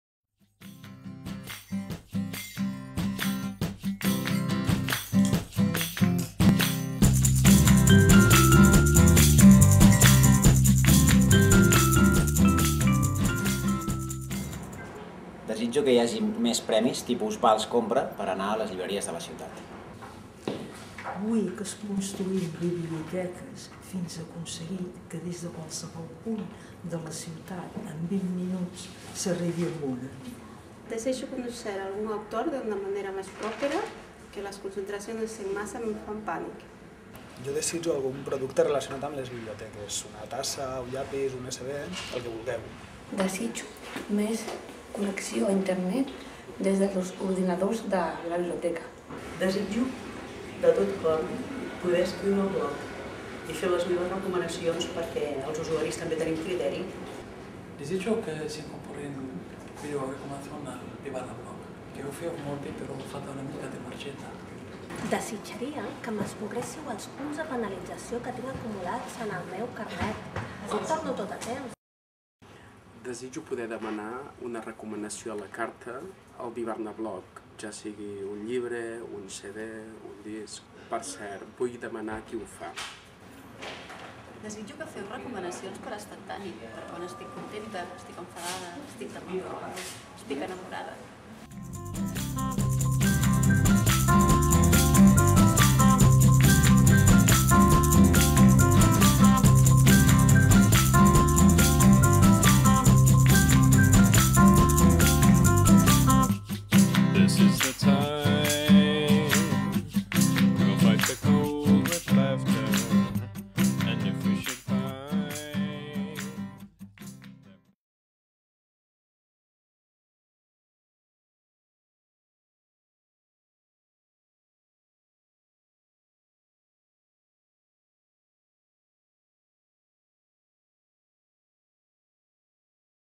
Aquest Nadal els Reis Mags del Bibarnabloc us concedeixen tres desitjos
Aquest any des del Bibarnabloc us volem concedir tres desitjos. Què demaneu?